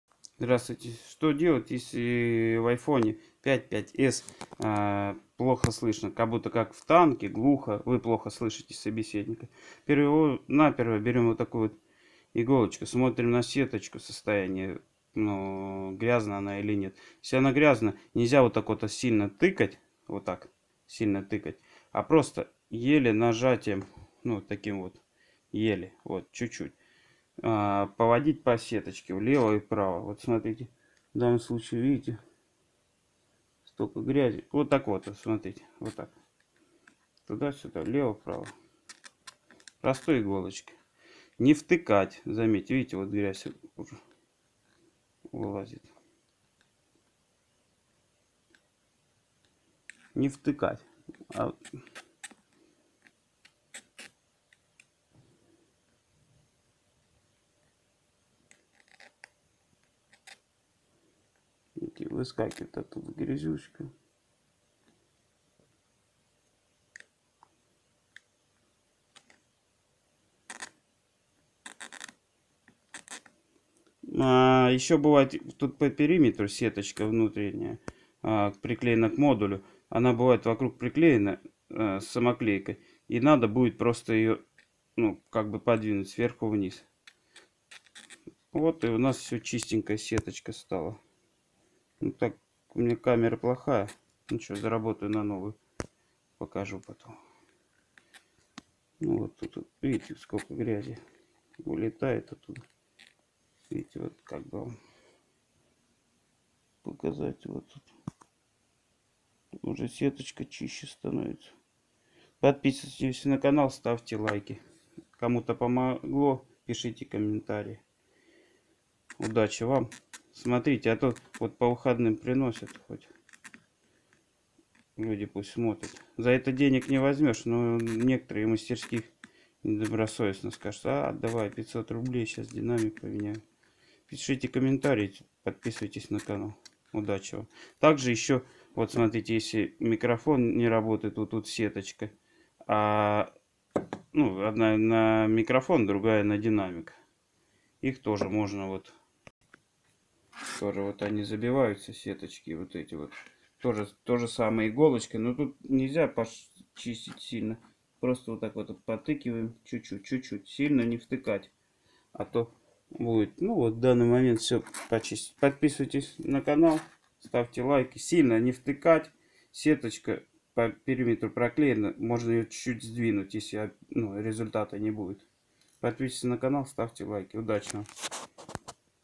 Что делать, если в iPhone слабый звук слухового динамика
Чистка динамика от пыли